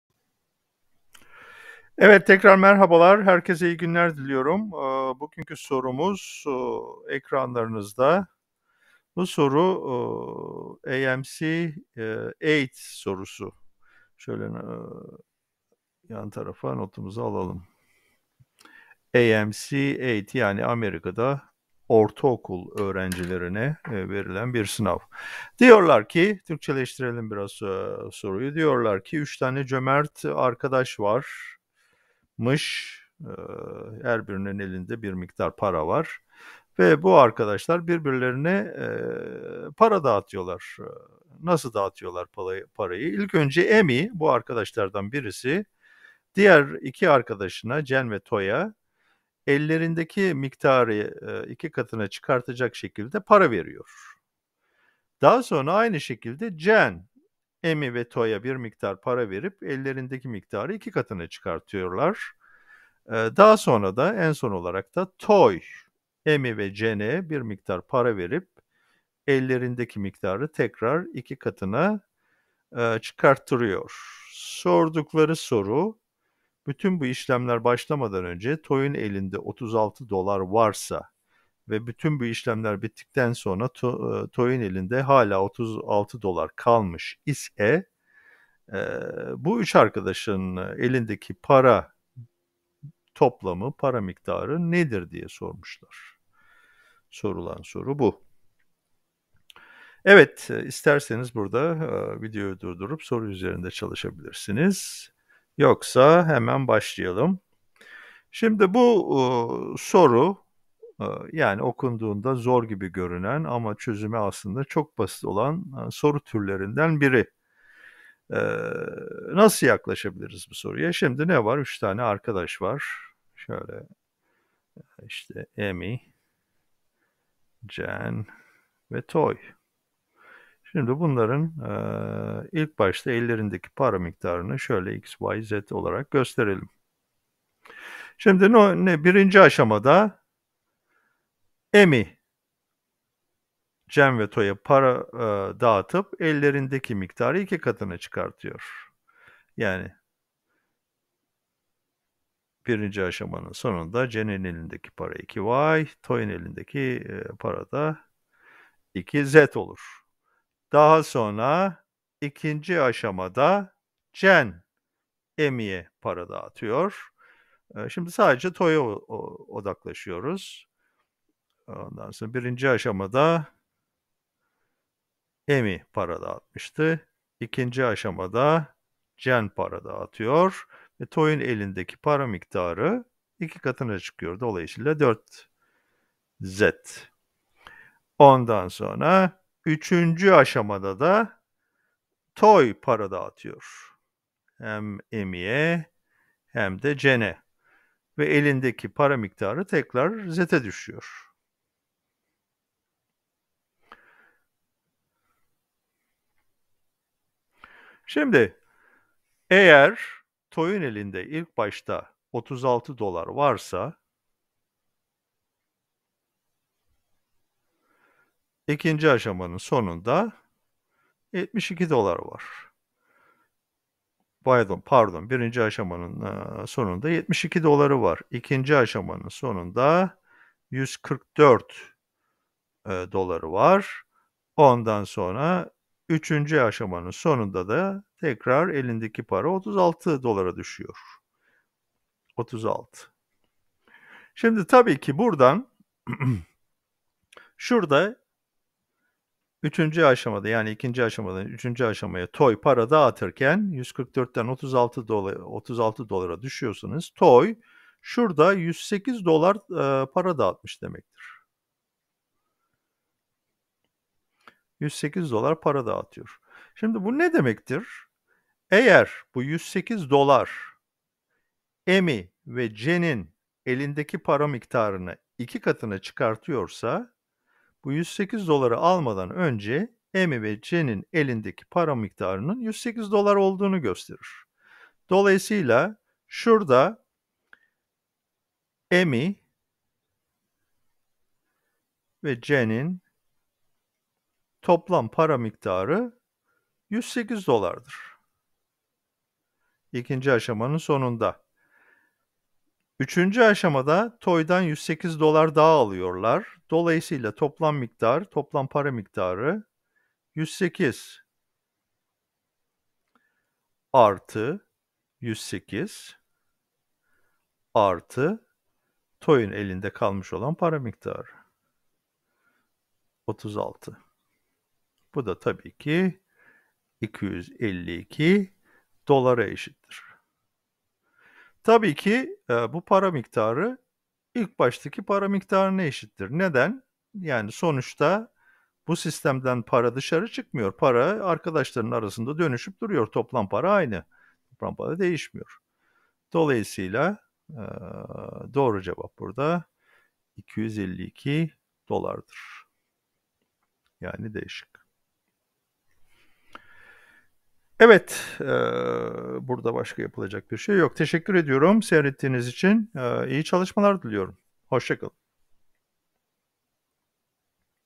AMC 8 Sorusu
Bugun gecmis yillarda sorulmus olan bir AMC 8 sorusu cozuyoruz.